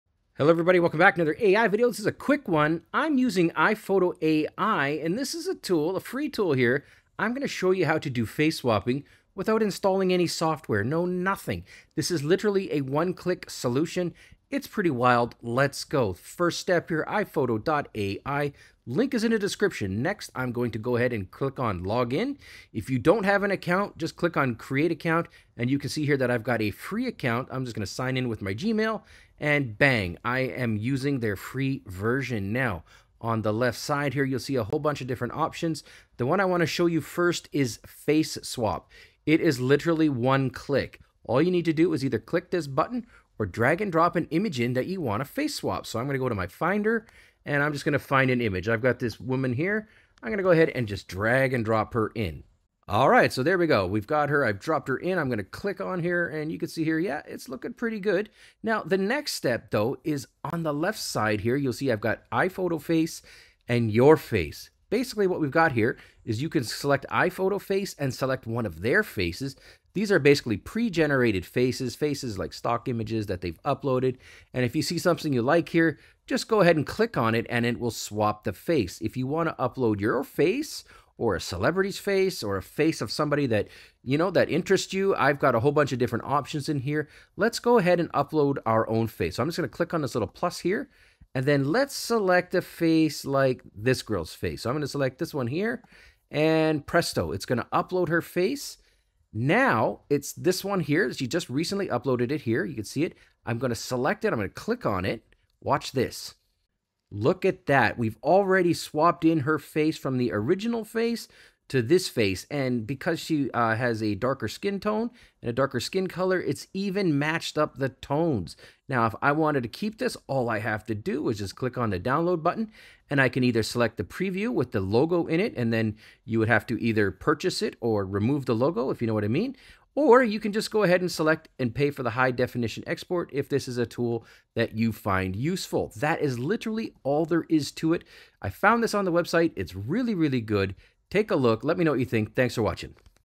Face Swapping with AI - No Software Needed! Easy Tutorial
Welcome back to the channel! 🌟 Curtis Pyke here with a quick and powerful tutorial on how to use an amazing, FREE AI tool called iFotoAI for effortless face swapping—all without having to install any software!

In this tutorial, I’ll walk you through:

✅ How to access and use AI Photo A AI
✅ Simple steps to log in or create a new account
✅ A live demo of performing a face swap with just one click
✅ Tips on how to choose from pre-generated faces or upload your own
✅ Finalizing your face swap and downloading options
Whether you’re a beginner or an experienced user, this tool makes face swapping as easy as a single click. Join me as I explore all the functionalities this free tool has to offer!

⏰ Timestamps:
00:00 - Introduction
00:20 - Setting Up the Account
00:36 - Starting the Face Swap
00:53 - Performing the Face Swap
01:10 - Exploring Additional Options
01:28 - Uploading and Selecting a New Face
01:45 - Finalizing the Face Swap
02:10 - Downloading the Result

This tool not only simplifies the face swapping process but also ensures high-quality results with skin tone matching. It's perfect for creating fun edits, experimenting with different looks, or just learning more about AI technology in image editing.

Your support means a lot and helps me bring more tutorials like this to you!

Have you tried AI Photo A AI? What are your thoughts on its functionality? Any other tutorials you'd like to see on the channel?